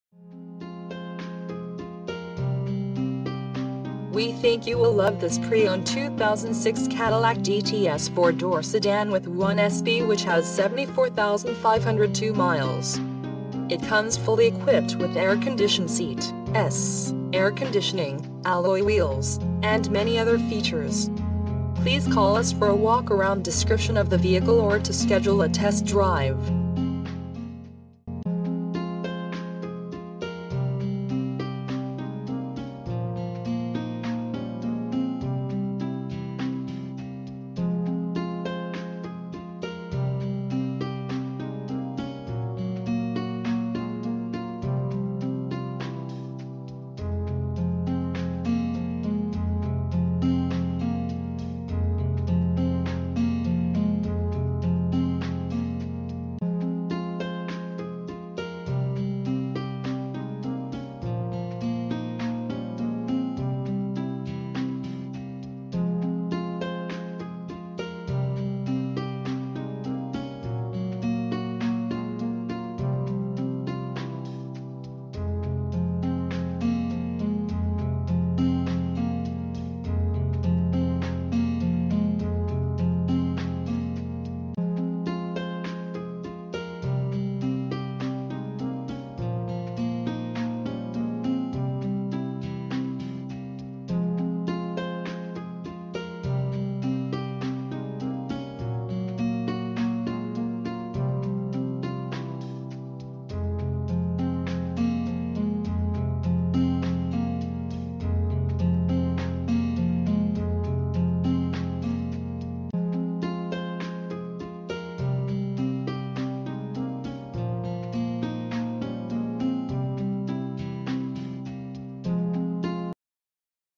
2006 Cadillac DTS 4dr Sdn w/1SB DUAL ZONE CLIMATE CONTROL TRACTION CONTROL
2006 Cadillac DTS 4 Door Sedan w/1SB
Stock Number: 2276R
VIN: 1G6KD57Y36U222879

Calvary Cars and Service
Norfolk, VA

We think you will love this PRE-OWNED 2006 Cadillac DTS 4 Door Sedan w/1SB which has 74502 miles. It comes fully equipped with Air Conditioned Seat(S), Air Conditioning, Alloy Wheels, and many other features.Please call us for a walk around description of the vehicle or to schedule a test drive.